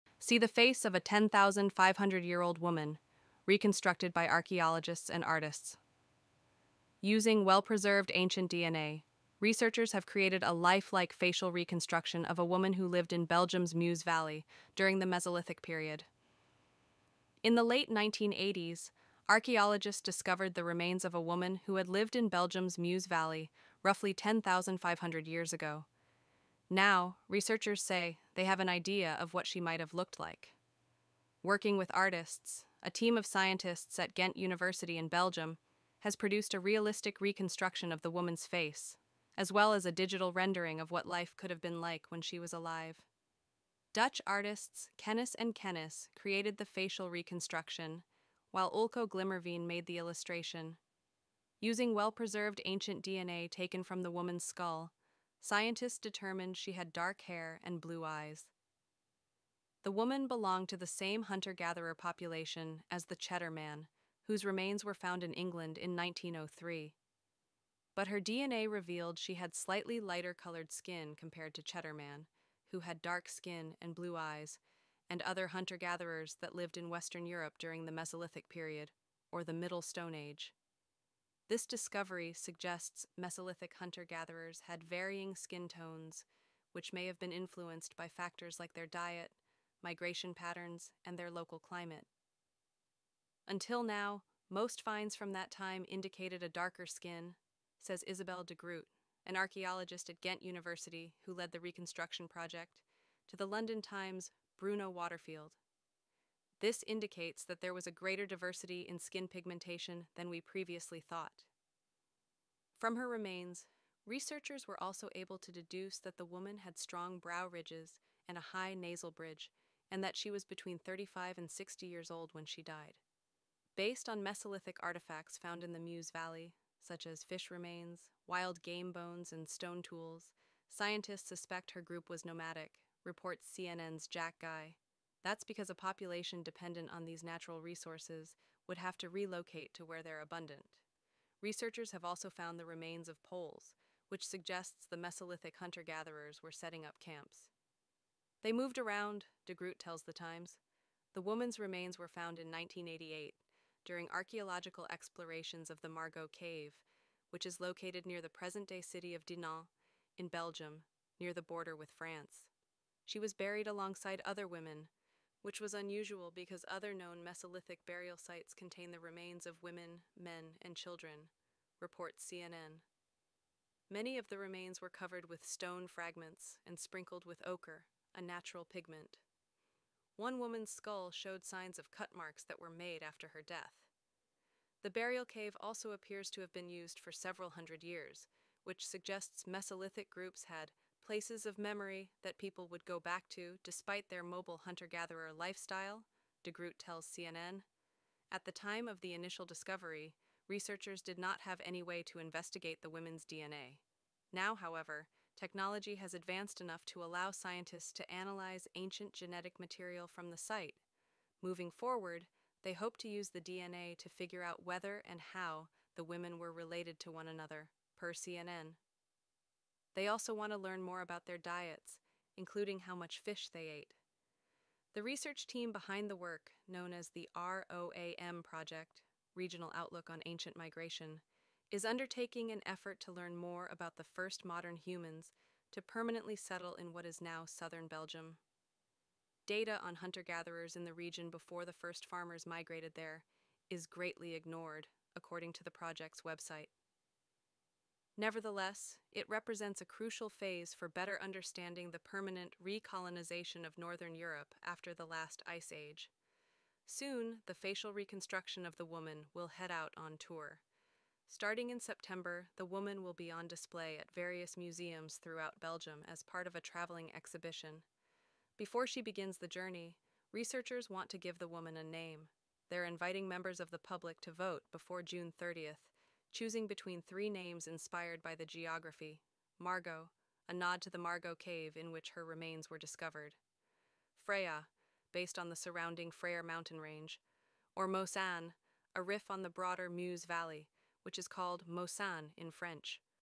See the Face of a 10,500-Year-Old Woman, Reconstructed by Archaeologists and Artists
Using well-preserved ancient DNA, researchers have created a life-like facial reconstruction of a woman who lived in Belgium’s Meuse Valley during the Mesolithic period
In the late 1980s, archaeologists discovered the remains of a woman who had lived in Belgium’s Meuse Valley roughly 10,500 years ago. Now, researchers say they have an idea of what she might have looked like.